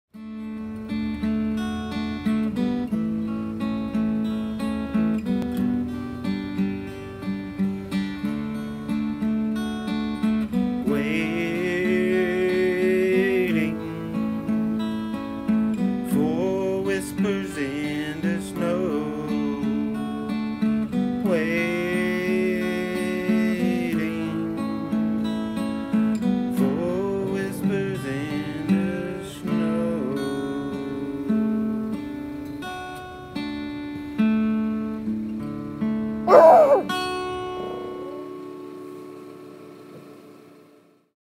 Whispers In The Snow
Hey everyone this is Mark Lawson, aka MaJel, with a special promotional, from my books in the Greystone Series. To answer the questions, yes it is I twirling the stick, singing, and playing the guitar. Hope you enjoy and download all of my books in the Series. Thank You, and God Bless.